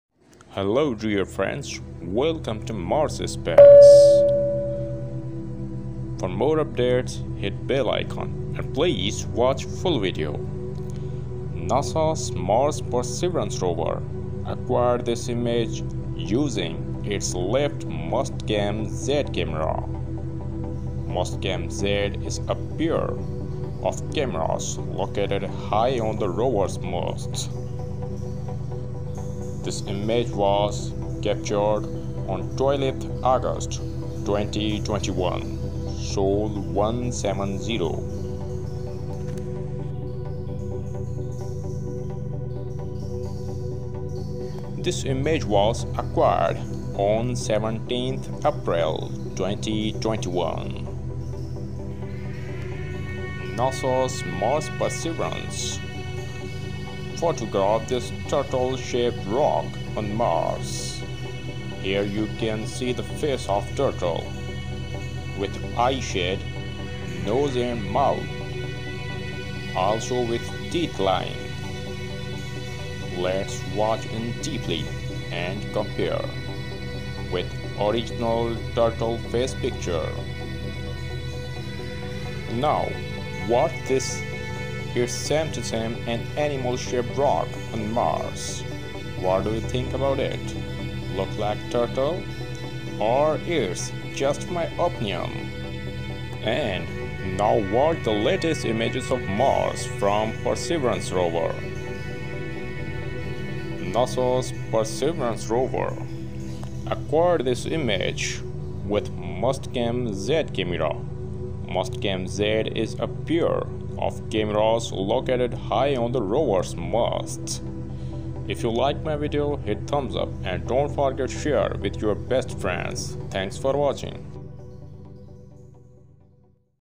Martian Turtle: Life on Mars | Turtle found on Mars by Nasa's Perseverance Rover
Credit: nasa.gov, NASA/JPL-Caltech